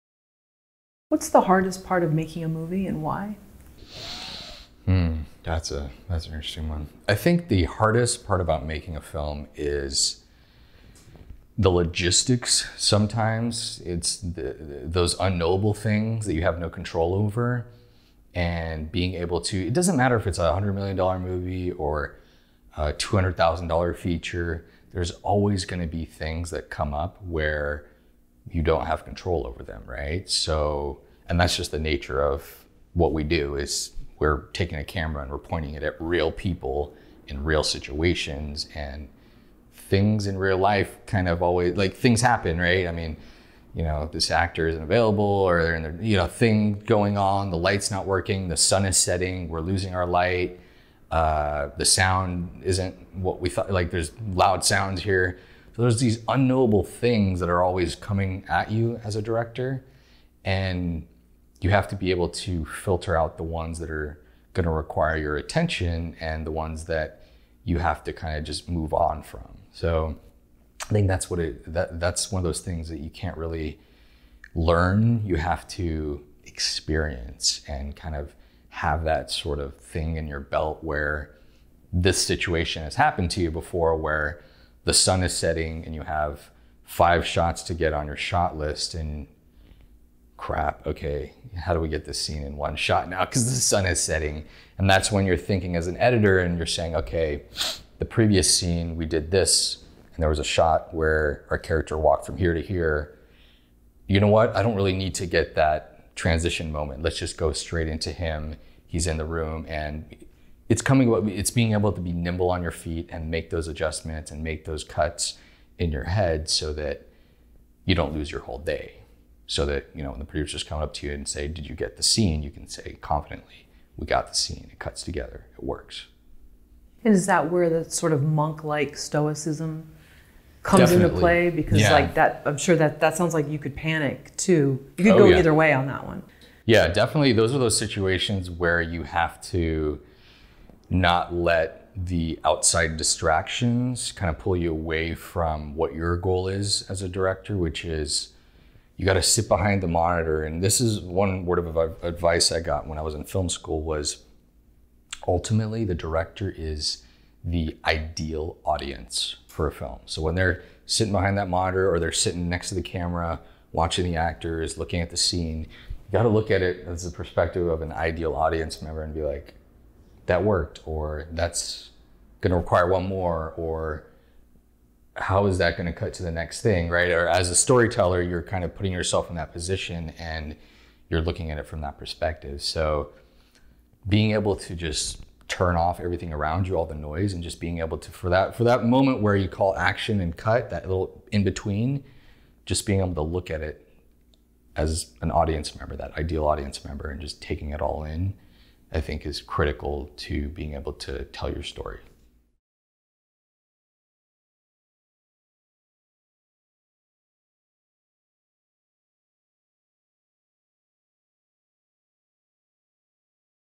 Hardest Part Of Making A Movie - Matt Eskandari
In this Film Courage video interview, Filmmaker Matt Eskandari on the Hardest Part Of Making A Movie.

AUDIO